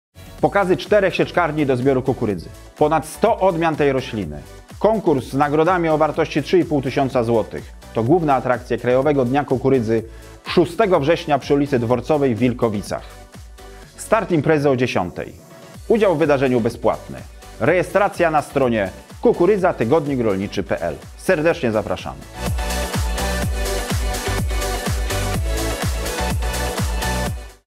Zobacz na żywo jak pracują sieczkarnie na Krajowym Dniu Kukurydzy 2022
👉Jak ograniczyć nakłady na uprawy kukurydzy? 👉 Jak nawozić kukurydzę przy wysokich cenach nawozów? 👉 Pokazy pracy sieczkarni 👉 100 odmian kukurydzy na kiszonkę i ziarno
Zobacz to na żywo podczas Krajowego Dnia Kukurydzy już 6 września 2022 r. w Wilkowicach pod Lesznem. zapisz się już teraz tutaj